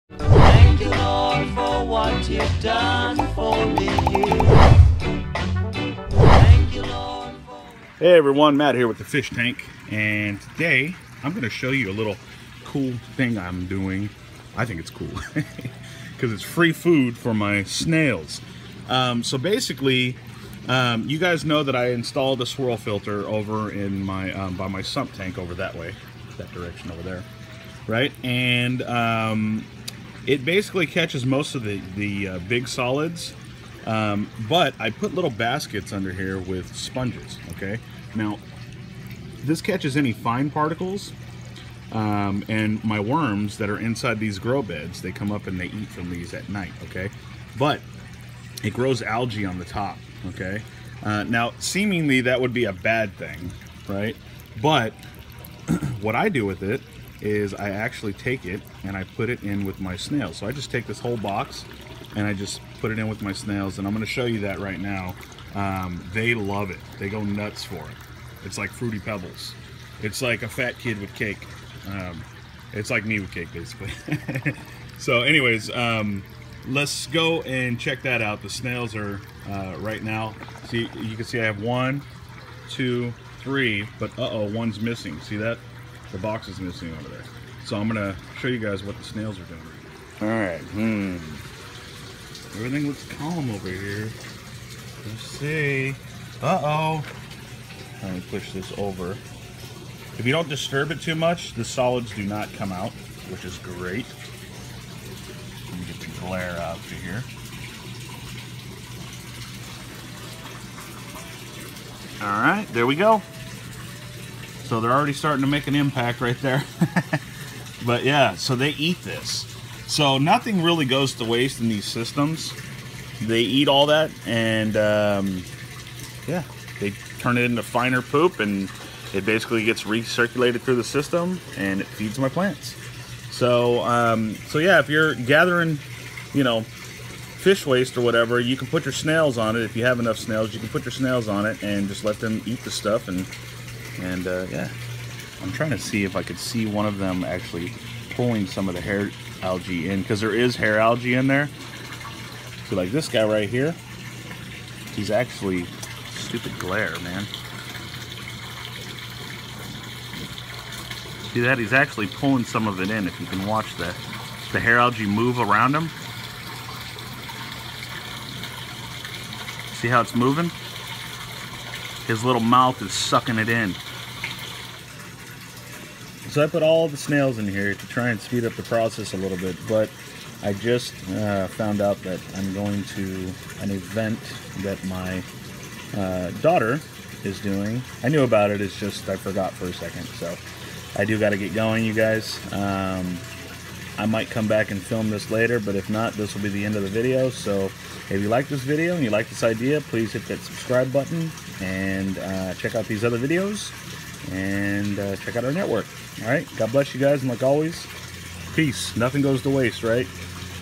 Snails Cleaning My Aquaponic System
These Apple/Mystery Snails do a really great job at cleaning up algae and and long hair algae! I used to have so much growing in this tank in my aquaponics tank but now that the snails are there 0 issues so I love them! They even clean off filters for me so nothing goes to waste:)